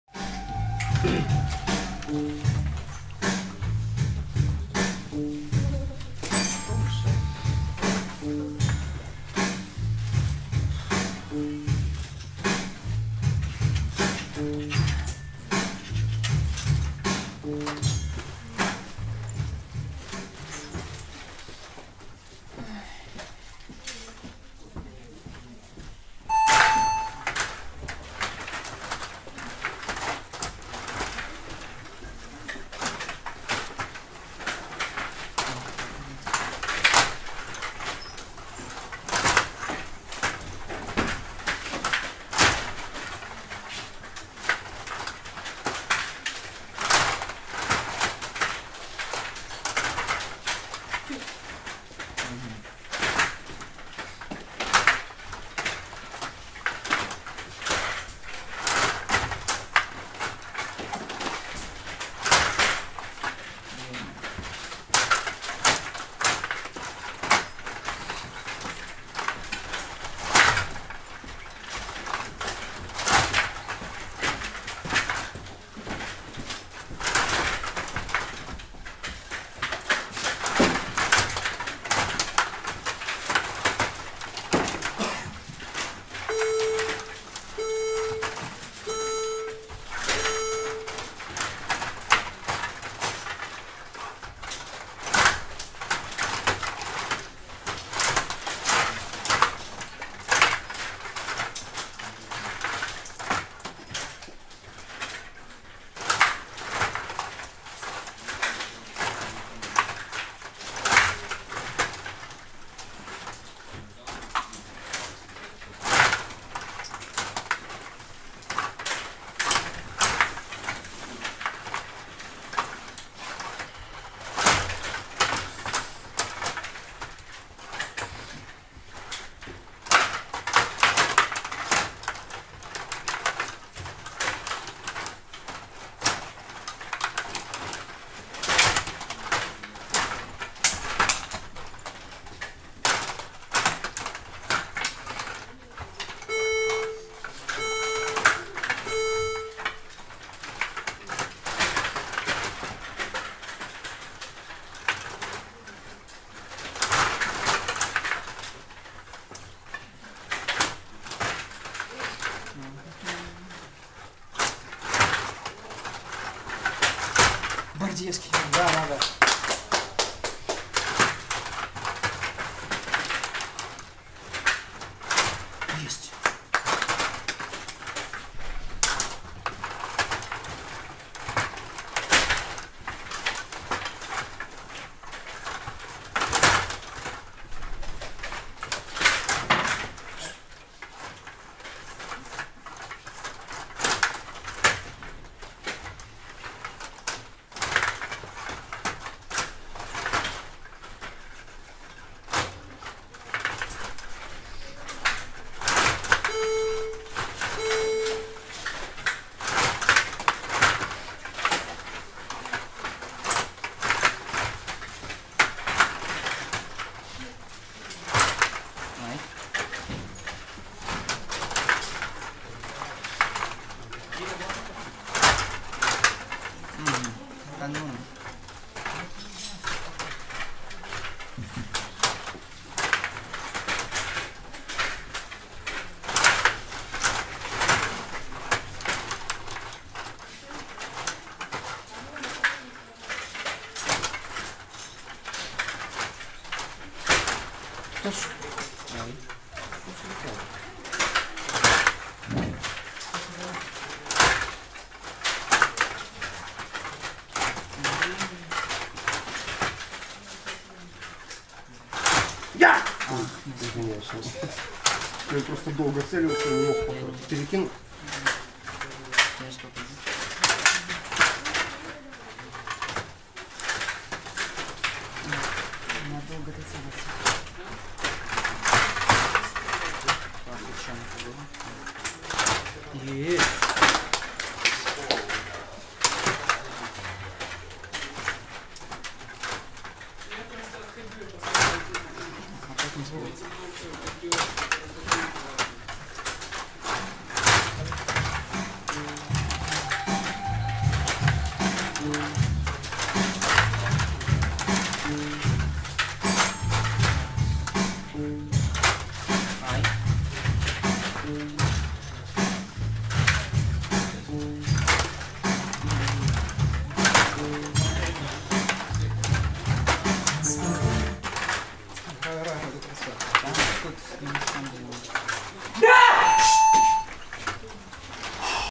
Urkainian Championship 2016 (3rd Round): Ignatenko Roman (UKR) - Sapozhnikov Vitaliy (UKR)
Ignatenko Roman (UKR) - Sapozhnikov Vitaliy (UKR)
Playoffs - Semi-Final
2nd Round
Ukrainian Championship 2016, 3rd Round (Kharkiv, Ukraine)